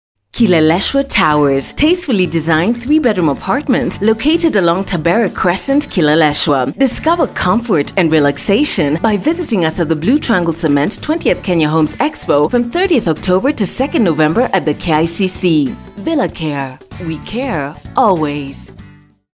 KILELESHWA TOWERS
Luxurious living within your reach. Come and learn more about this development at the 20h Kenya homes Expo at the KICC from the 30th of October 2014